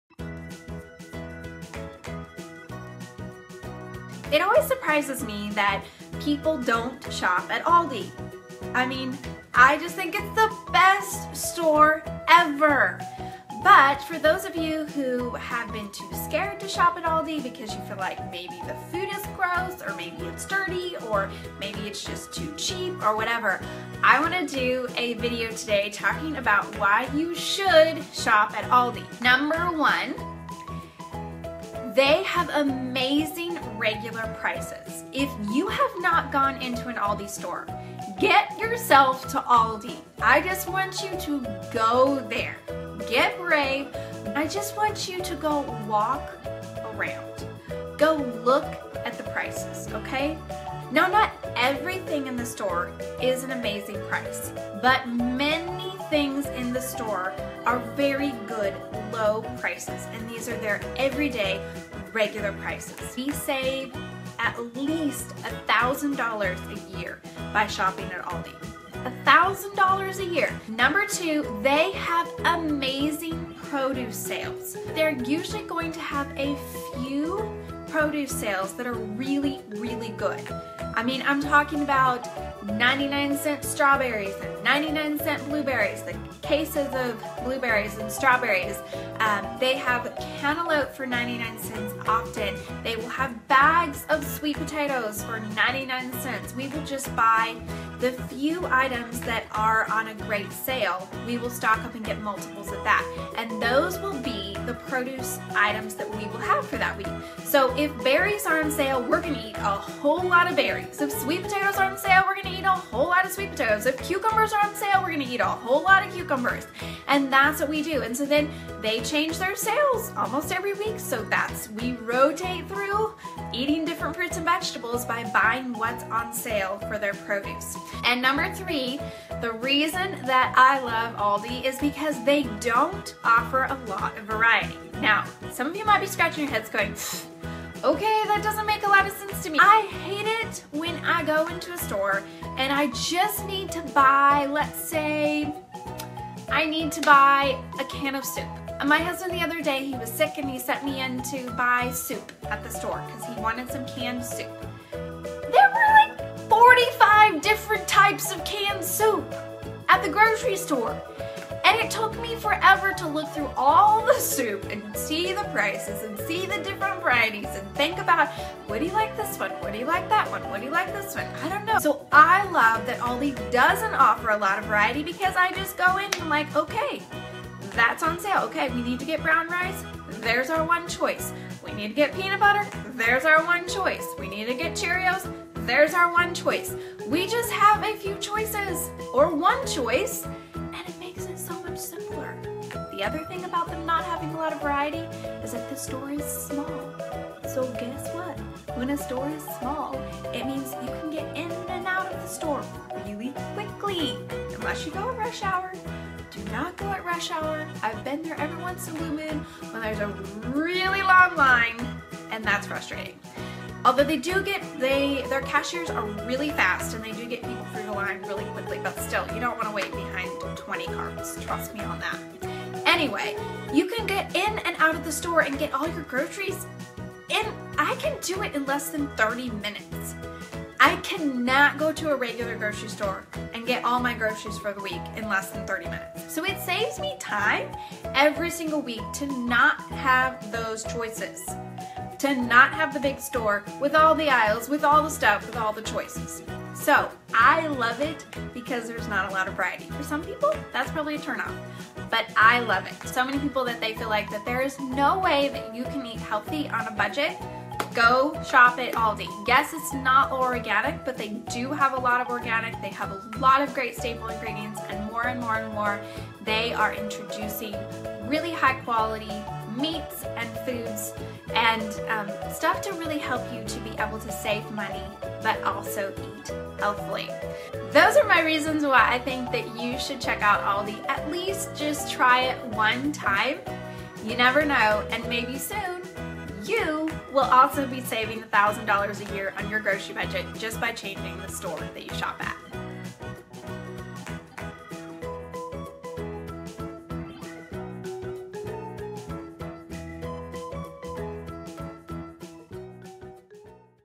Why You Should Shop At ALDI
We save so much money by shopping at ALDI! Here are 3 reasons I love ALDI & why I think you should try shopping there, too!

Read more about shopping at ALDI & how to save money there